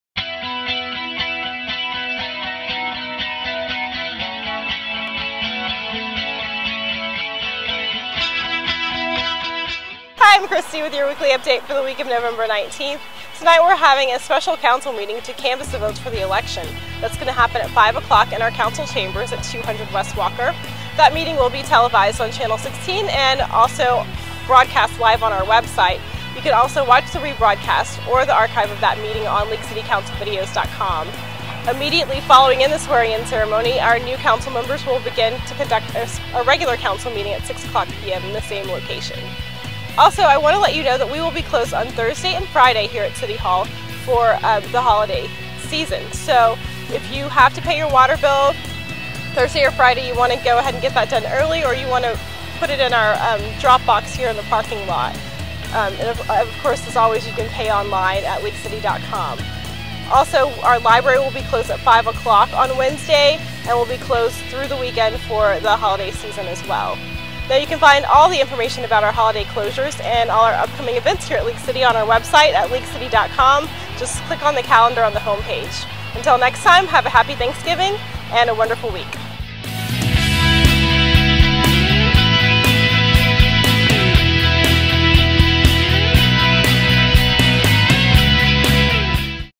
Weekly Update 11-19
Tonight there is a special meeting at 5pm to canvas the election returns and swear in the incoming council members. The City of league City will be closed on Thursday and Friday in observance of the Thanksgiving holiday.